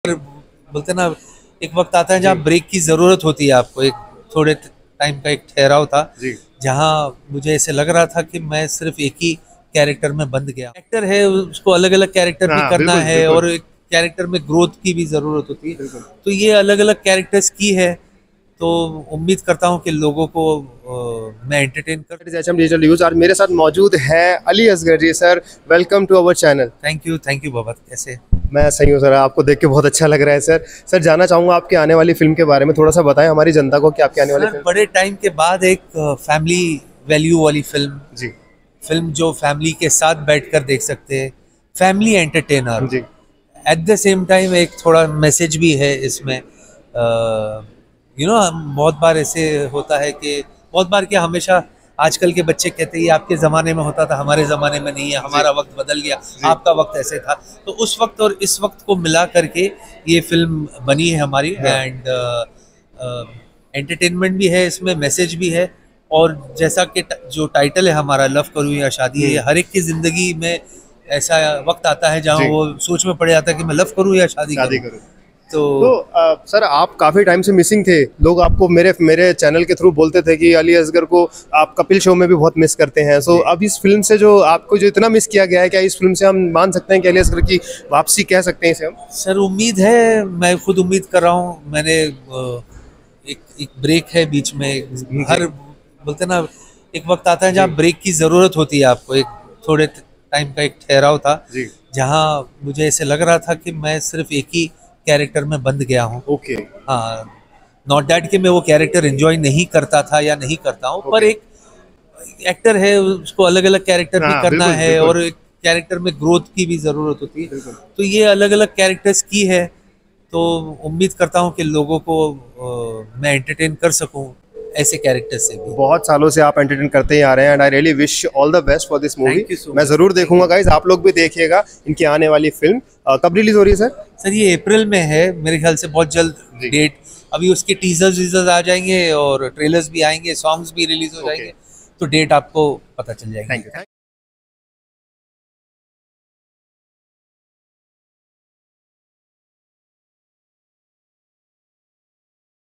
Actor 'Ali Asgar' reveals TRUE REASON behind leaving 'THE KAPIL SHARMA SHOW'' || Exclusive Interview
Today, at the promotion of his upcoming & comeback movie "Love Karu Ya Shaadi", actor Ali Asgar shared true reason behind leaving TV, Bollywood & The Kapil Sharma Show for long.